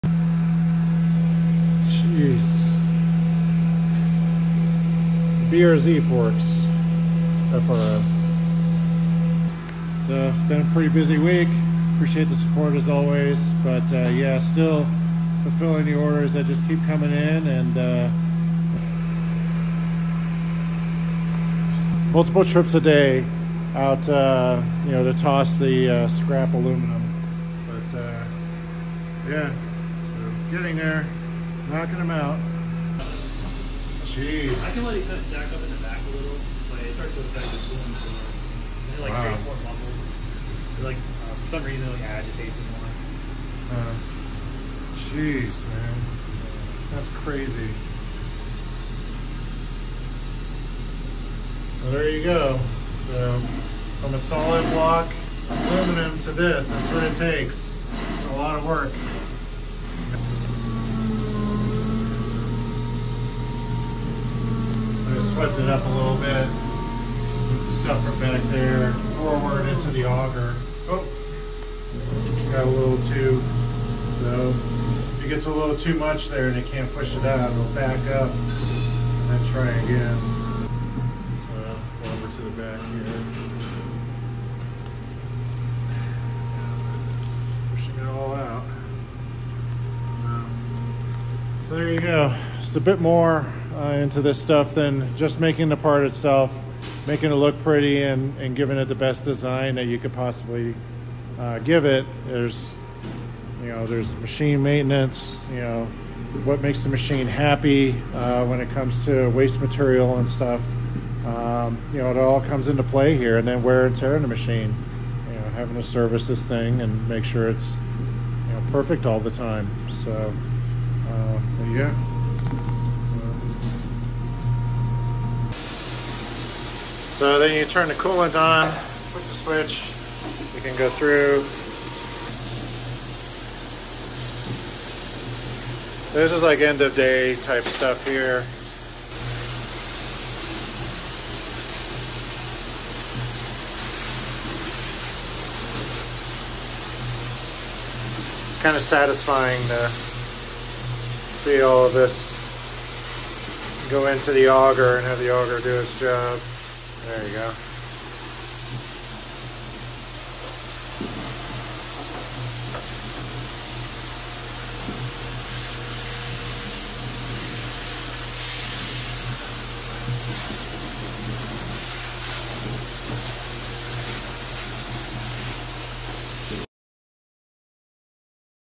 CNC Working Overtime Fulfilling Large Numbers of Parts Orders Made In-House at Jacks Transmissions
Trying to run the machine more efficiently by adding 2 or 3 more vises to the process.  More parts are done faster at once which allows for a huge amount of aluminum chips to pile up all over the place.  Sharing that part of things here.

The bits the machine uses can clog the unit with material, and running too many vises in one shot can also cause a problem of too much waste material.  Everything is a balance to make a superior part yet allow the machine to be happy and do its job well and consistently.

Our parts are made right here in the USA in-house at Jacks Transmissions!  We design and build them ourselves!